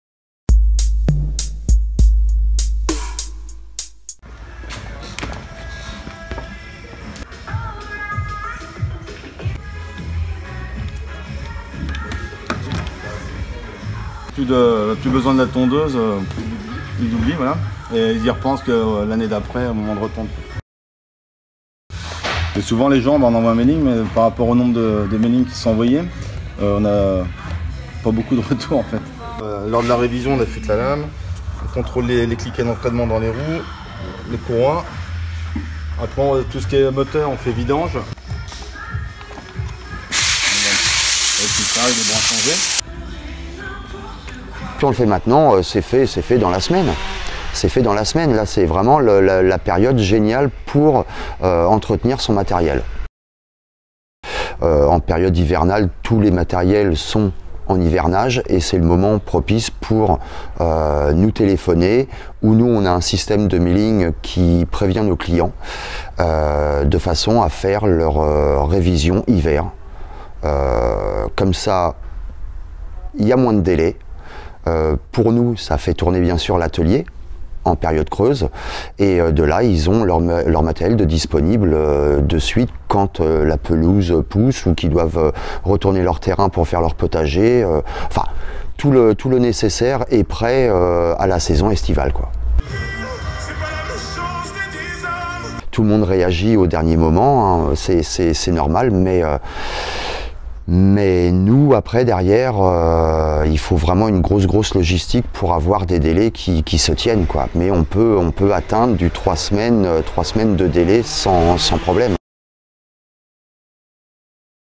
VIDEO. Loir-et-Cher : c’est le moment de faire réviser votre tondeuse
La saison estivale est terminée. N’attendez pas le printemps pour emmener votre tondeuse à la révision. Faites-le dès maintenant.